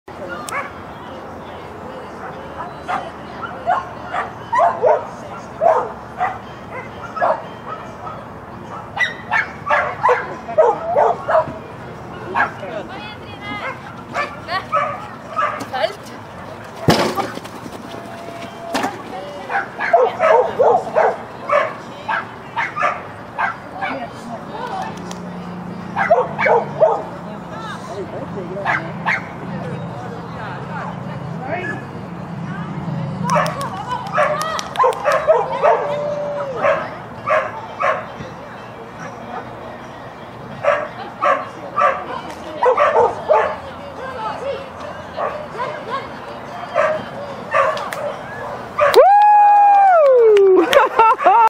My Schapendoes Dog Rover & Trine Thorkildsen, Clean Agility Run & 6th Place
My Schapendoes dog Rover & Trine Thorkildsen got a clean agility run & 6th place in Stavanger, May 1, 2016
Thank you, Trine!